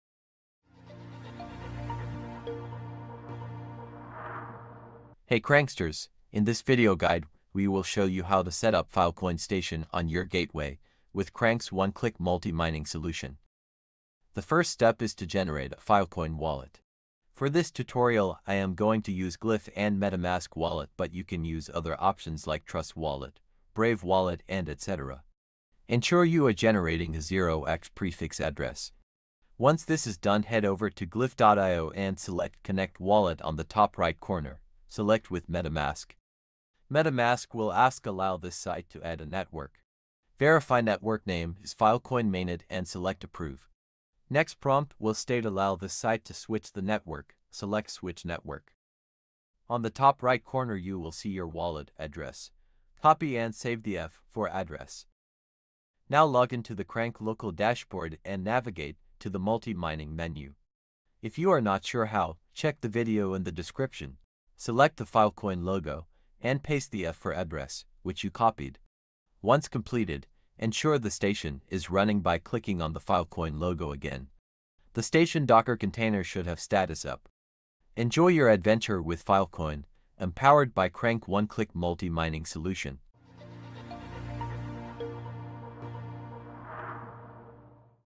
How to Set-Up Filecoin with Crankk Multimining!
Crankk.io is a full IoT Proof-Of-Network-Participation Crypto Network, proudly built on Kadena.

This video guide provides a step-by-step instructions on how to set-up Filecoin on your Gateway with Crankk's One-Click Multimining Solution!
Dive into the world of Multimining and be part of our Community!

Let's Crankk it Up!